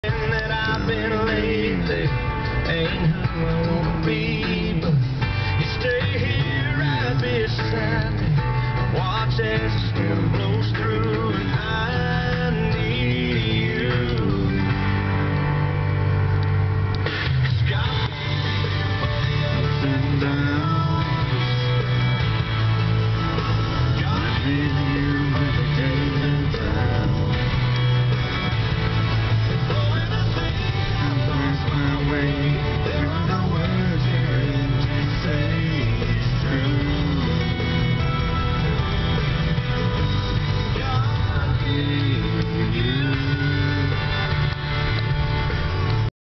Whiting in December
Traveling down 119th St. (Main street) in Whiting, Indiana.